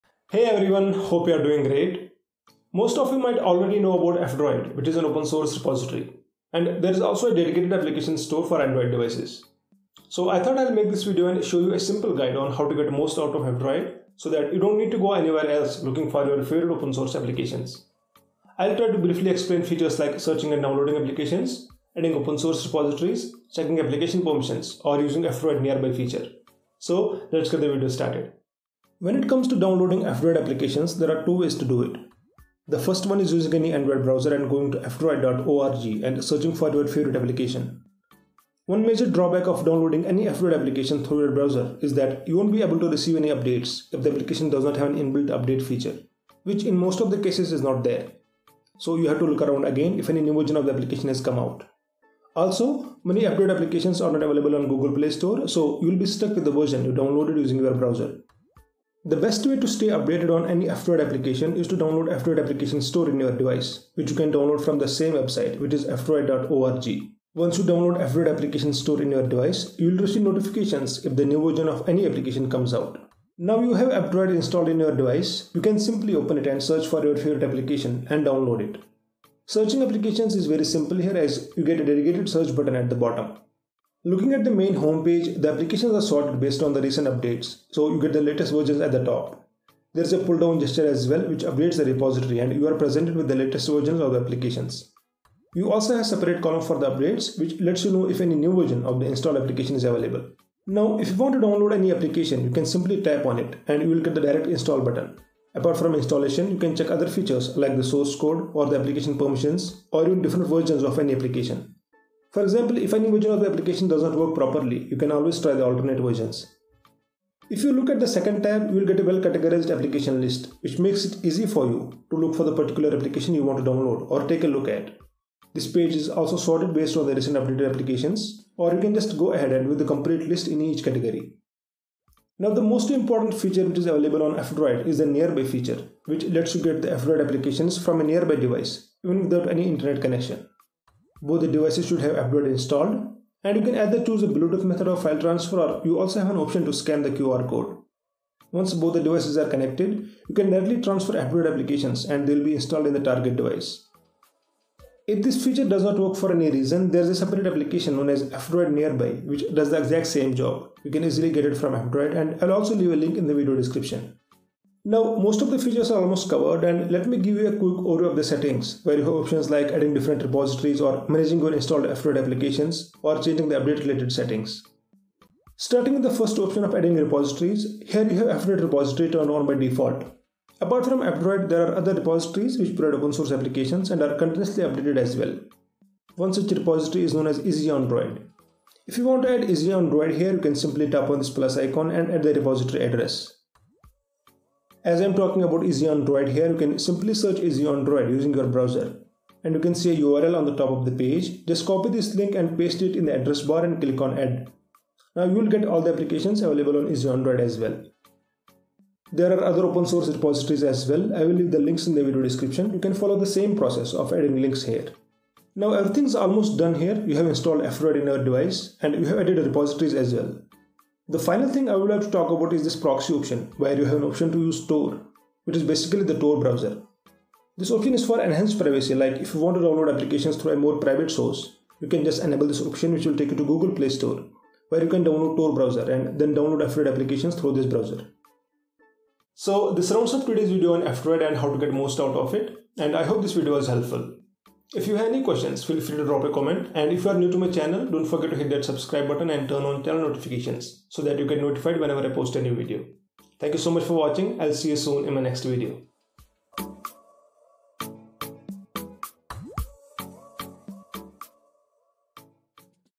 F-Droid Guide | F-Droid App Store
This video is a simple guide on how to use f-droid app store in your android device. F-droid is an opensource repository which lets you browse and install opensource apps in your android device.

You can add these opensource repos to F-Droid